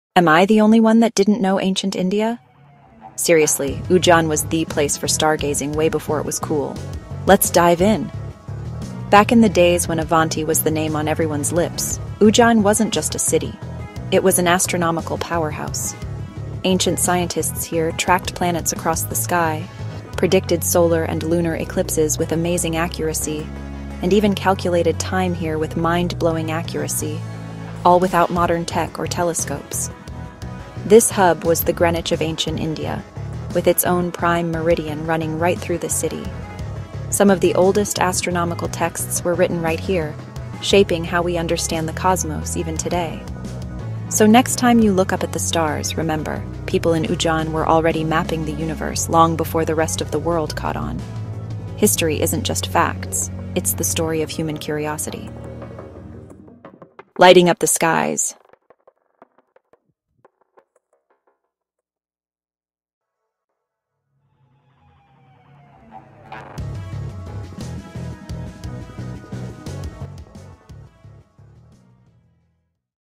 How Ujjain Mapped the Stars Before Telescopes!
Discover how Ujjain — ancient Avanti — became India’s astronomical powerhouse long before telescopes. In 45 seconds we bring history alive: the Ujjain Astronomical Center, its meridian traditions, star mapping techniques, and how Sanskrit scholars tracked equinoxes and celestial cycles using instruments like gnomons and armillary concepts. Perfect for history buffs, archaeoastronomy fans, and curious travelers. Learn why Ujjain was a prime reference point for medieval and classical sky charts and how those observations influenced navigation, calendars, and astrology across South Asia.

OUTLINE:

00:00:00 The Ujjain Astronomical Legacy